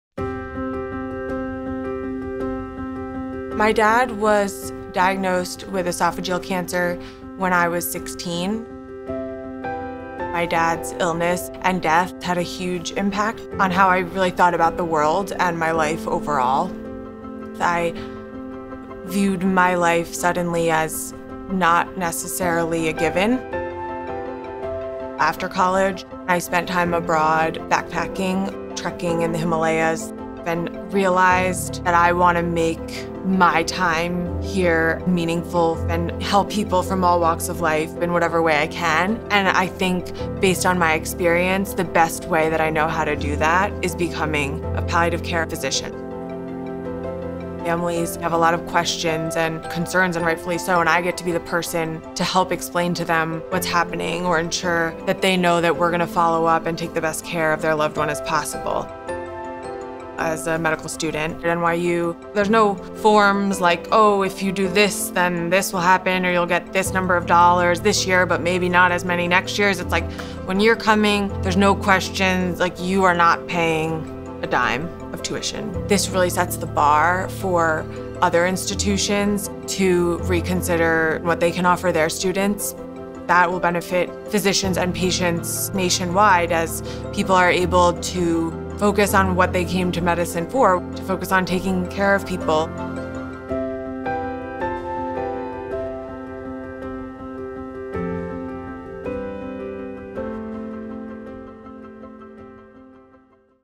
Tuition Free One-Year Anniversary (Gwen Cody)
NYU School of Medicine student Gwen Cody ’20 reflects on our tuition free initiative, her journey to medical school, and a belief that taking care of people is the best way she can make a meaningful contribution.